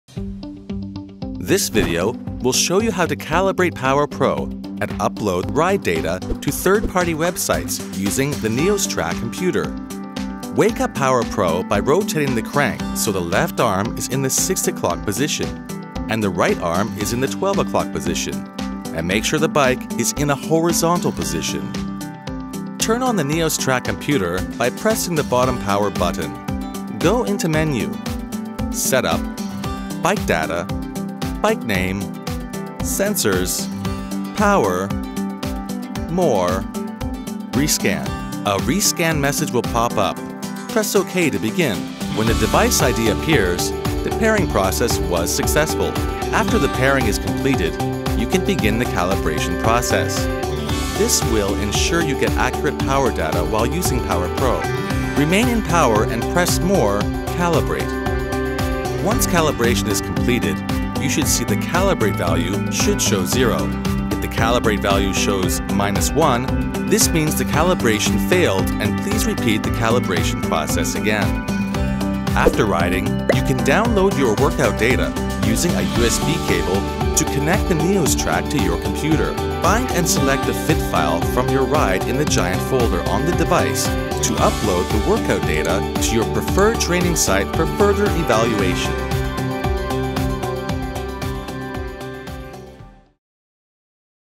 [자이언트] '파워 프로' 네오스트랙 속도계 활용 #3 / PowerPro
자이언트 파워 프로(Power Pro)를 NeosTrack 속도계와 페어링하여 PowerPro를 캘리브레이션하고 웹 사이트에 라이딩 데이터를 업로드 하는 방법을 안내합니다.

Power Pro에 대한 자세한 내용은 를 통해 확인 가능합니다.